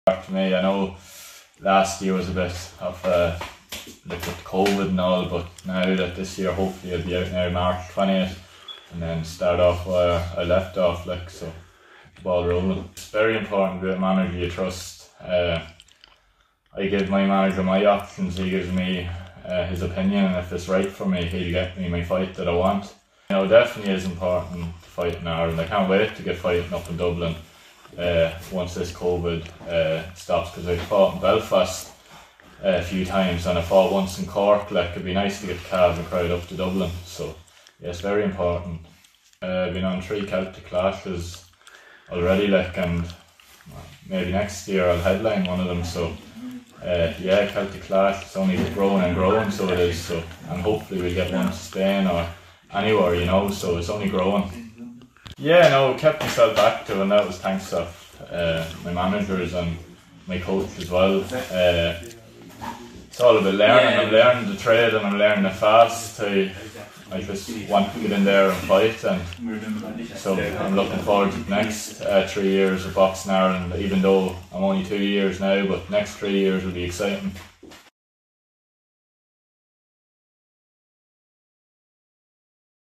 Cavan boxer Dominic Donegan talks about upcoming fight in Luxembourg
Cavan boxer Dominic Donegan talks about upcoming fight in Luxembourg and signing a new three year contract with Boxing Ireland Promotions